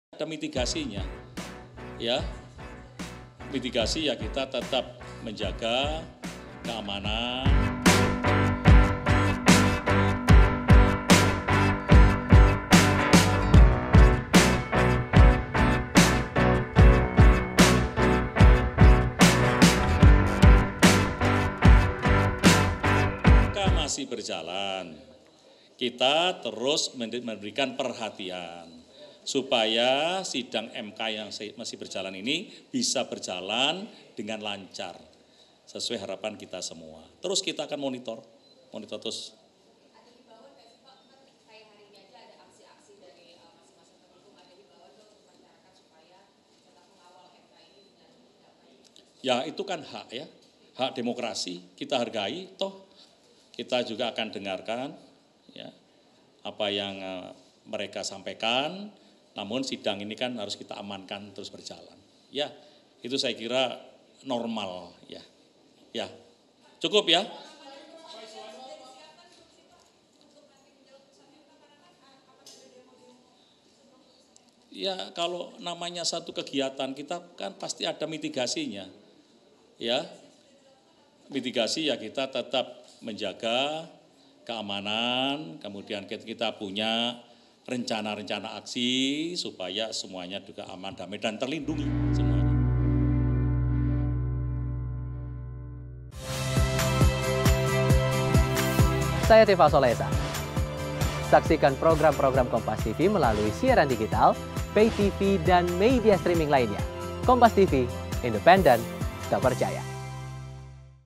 Antisipasi Hasil Sengketa Pemilu, Menko Polhukam Hadi Jabarkan Mitigasi Pengamanan Pilpres 2024
JAKARTA, KOMPAS.TV  Menko Polhukam RI, Hadi Tjahjanto mengatakan bahwa pihaknya telah menyiapkan langkah-langkah mitigasi untuk pengamanan jelang sidang putusan sengketa Pilpres 2024 di Mahkamah Konstitusi (MK).

"Ya kalau namanya suatu kegiatan kita kan pasti ada mitigasinya ya, mitigasi ya kita tetap menjaga keamanan," ujar Menko Polhukam RI, Hadi Tjahjanto pada Kamis, (18/4/2024).

Diketahui, MK akan bacakan putusan sidang sengketa Pilpres 2024 pada Senin, (22/4/2024) mendatang.